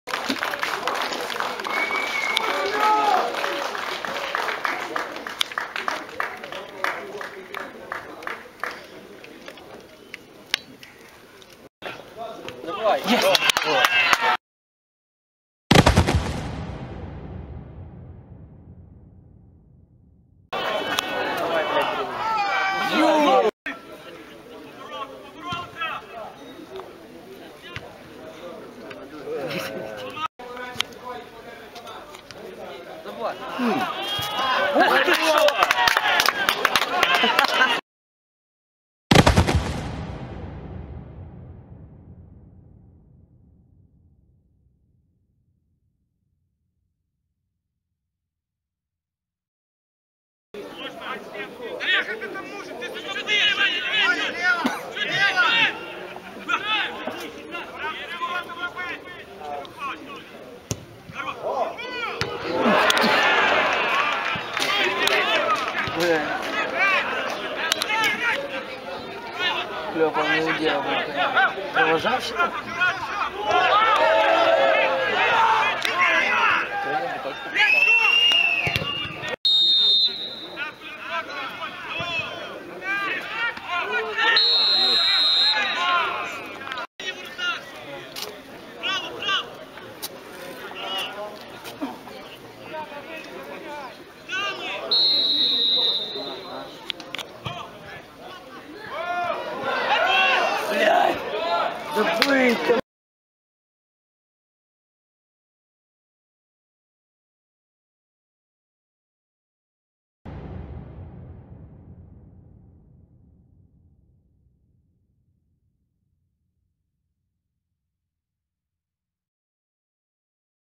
Случ Березне - ФК Гоща-АМАКО. Фрагменти + 2 голи. 13 тур. Чемпіонат Рівненщини 2010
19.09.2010, м. Березне, стадіон Колос, 1:3 (на відео голи Олександра Дмитрука на 3 хв і 20 хв). Детальний звіт про гру